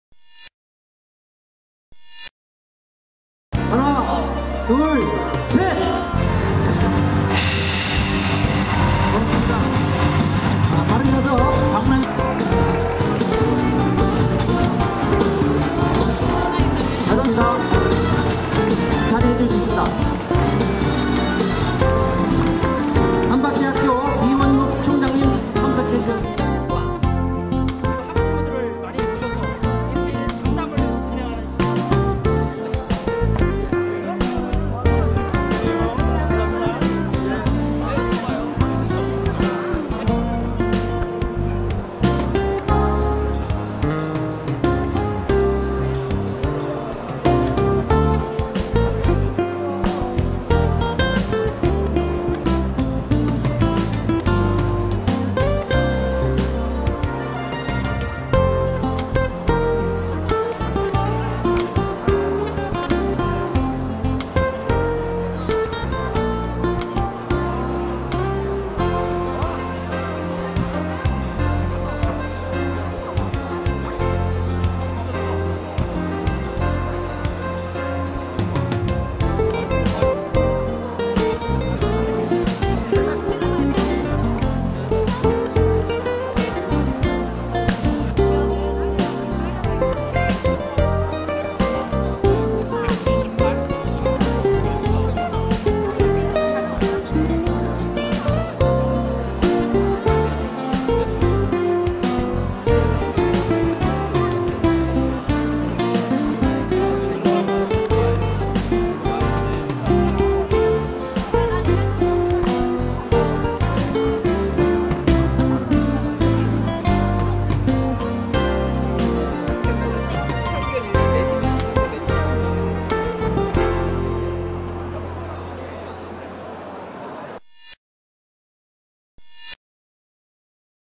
대학입시정보박람회
중도일보가 주최하는 중부권 최대의 대학입시박람회인'2013 대학입시 정보박람회'가 29일 대전무역전시관에서 개막됐다.

첫날에만 7000여명의 학생ㆍ학부모ㆍ 교사들이 박람회장을 찾아 각 대학의 다양한 입시전형 방식 등을 살펴봤다.